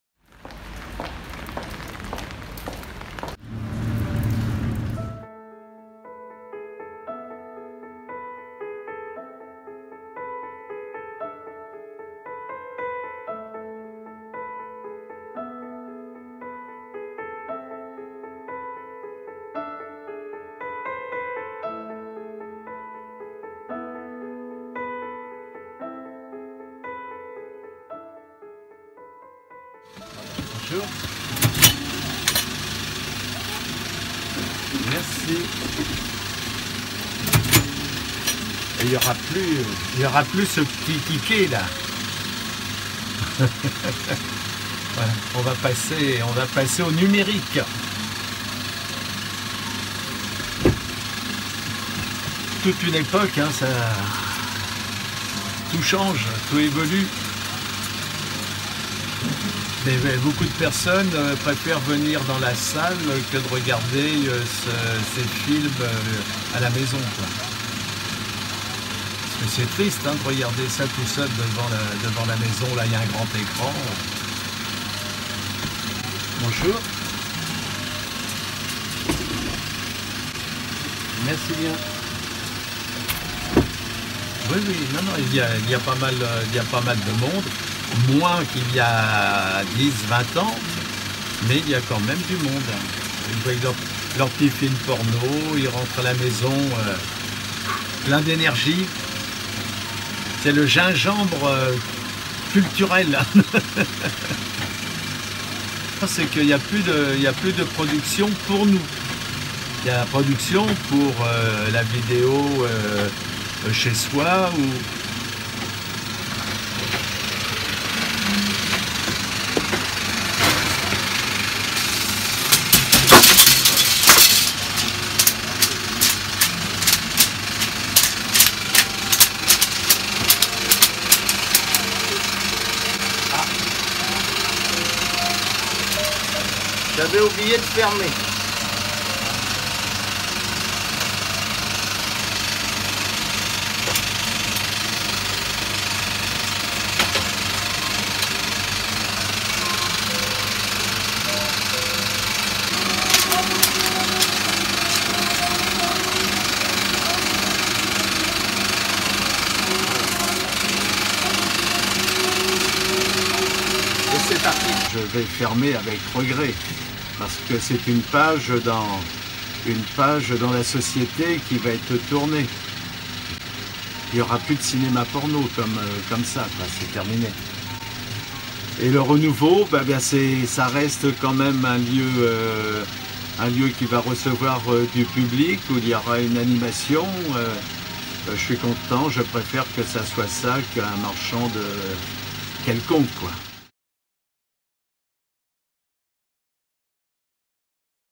“色情电影”是文化生活中的姜片”
【“色情电影”是文化生活中的姜片”】欧洲微视点#
巴黎最后一家专门播放色情电影的影院将于3月末关闭。
“一个时代结束”，影院老板拉罗什说，“之后影院由剧院接替，我算是开心。这里继续接待民众，举办活动，没有被商铺拿下”，“影院还是有客人的，相比二十年前，客人是少，但还是断断续续来客人。年轻人难以继续经营的原因有二：税务重，发行商不再发行为影院播放的色情电影。”
拉罗什的影院拥有100个座位和一个包厢，每周两次开放“情侣之夜”。拉罗什说，自己的工作没什么特别，电影分爱情电影、喜剧电影、枪战电影，…… 自己播放的只不过是其中的一种。“色情电影能让人精力焕发，可惜现在观众更愿意在家里观看，一个人对着大屏幕。”
关于开办影院最难忘的经历，拉罗什说，15年前，一对夫妇领着他们的孩子向他介绍，“这孩子是电影院里爱的结晶”。